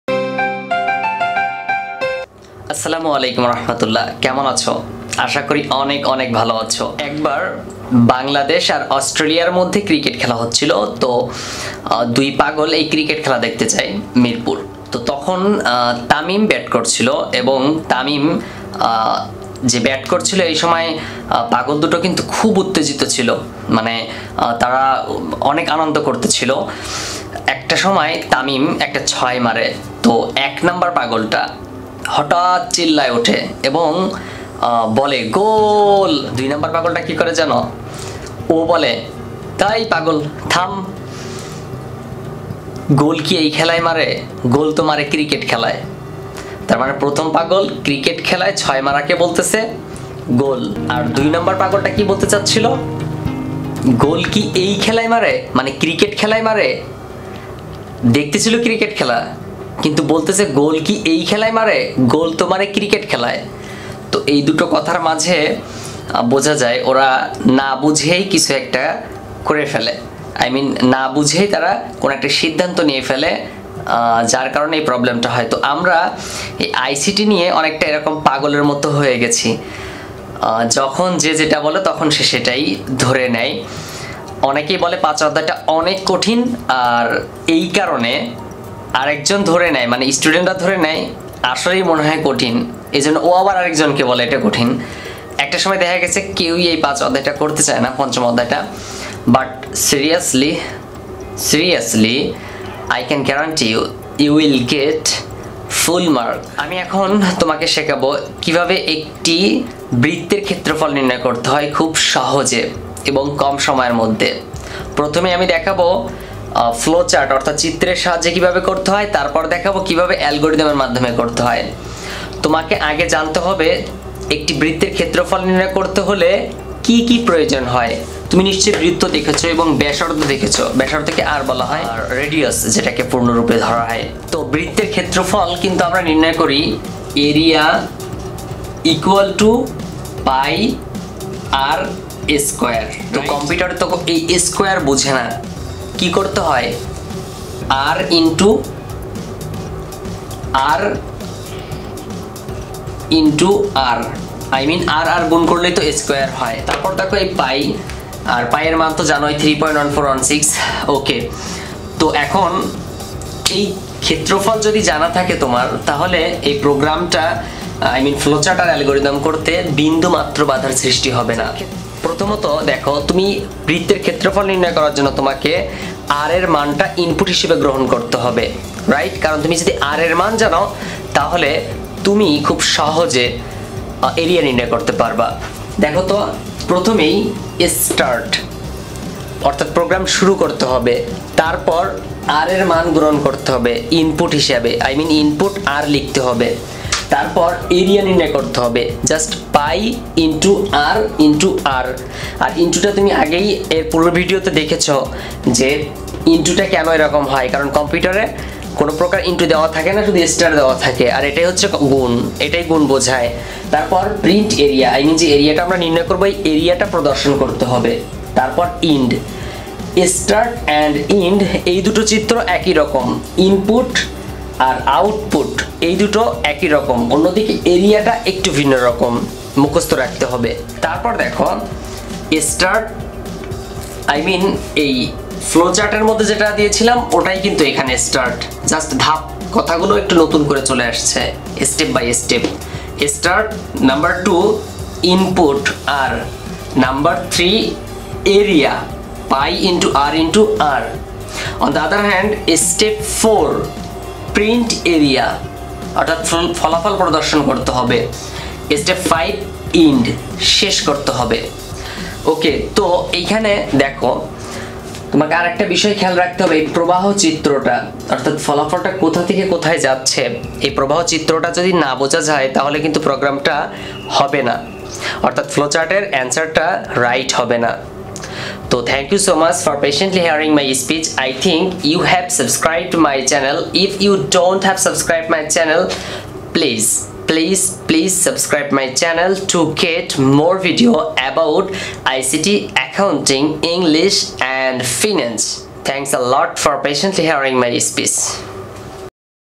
HSC ICT chapter 5.1 | Area of the Circle | Algorithm & Flow Chart | মনে রাখার সবচেয়ে সহজ পদ্ধতি
ICT বিষয়ের প্রতি কোন সমস্যা থাকলে আমাদেরর ফেসবুক পেজে নজর রাখুন।